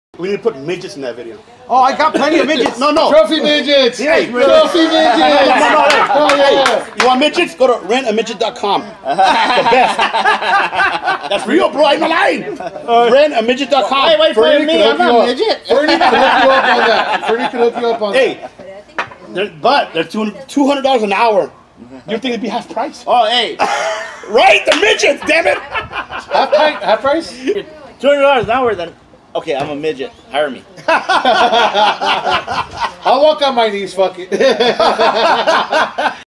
Rent-A-Midget.com (Keggercast Ep. 2 Extra)
Keggercast Episode 2 outtake: Fernie tells us about Rent-A-Midget.com and Hoser contemplates whether he is a midget or not.